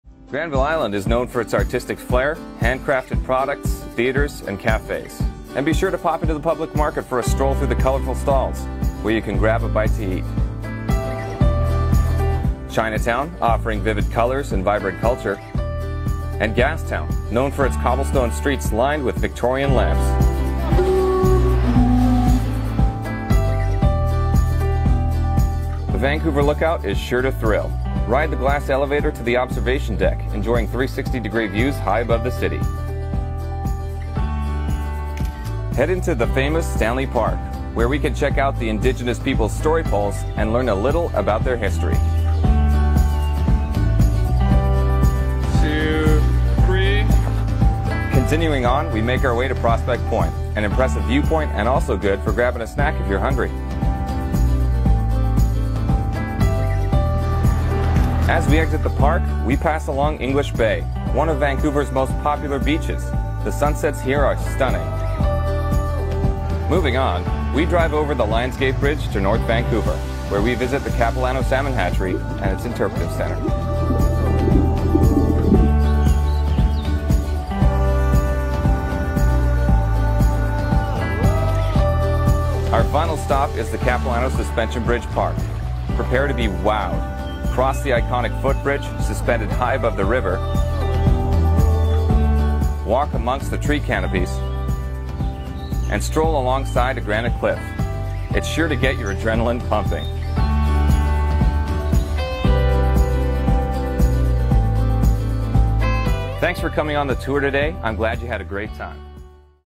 Vancouver City Tour, Capilano Suspension Bridge & Lookout Tower with Breakaway Experiences
To purchase Vancouver City Sightseeing Tour plus Capilano Suspension Bridge + Lookout Tower go

Explore the urban elements of downtown Vancouver and immerse yourself in the wilderness of Vancouver’s rugged North Shore. This unique Vancouver City sightseeing tour includes the Capilano Suspension Bridge and the Lookout Tower. It’s the perfect blend of city and nature in Vancouver.